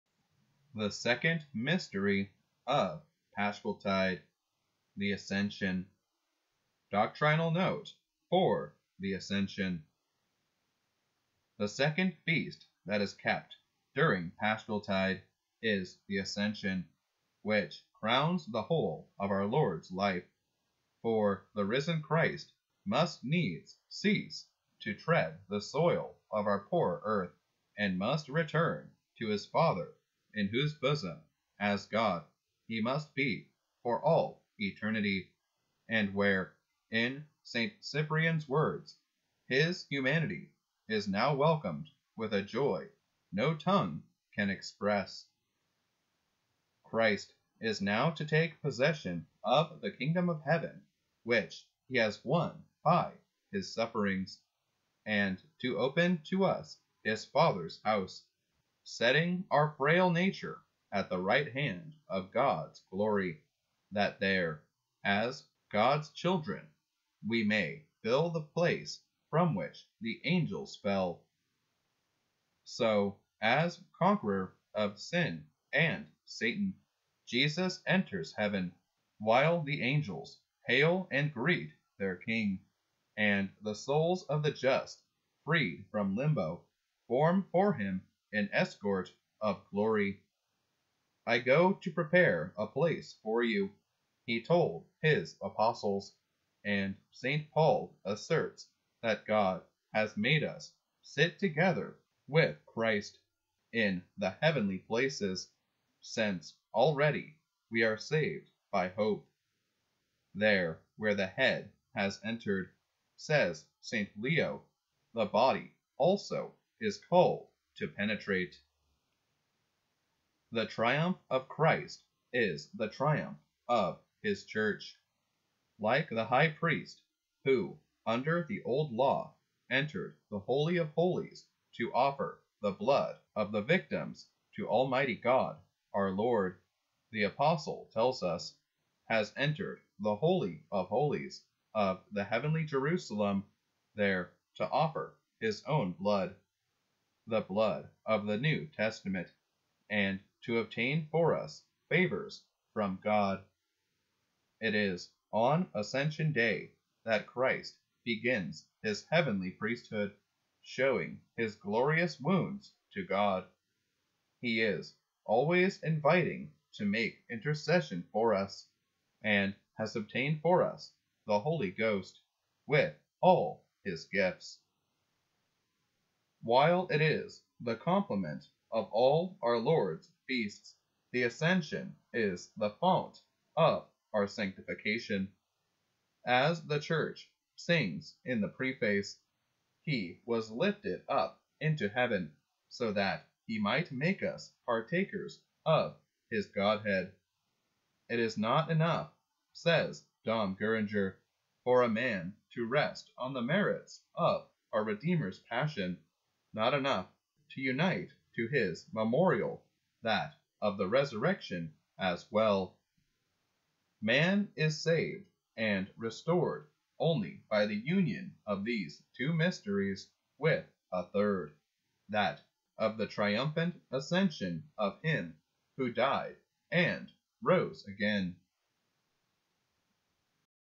The Ascension: Doctrinal Note
Part 1 of 3, on the Ascension. VRP Catholic Media 2023. Read from The St. Andrew Daily Missal.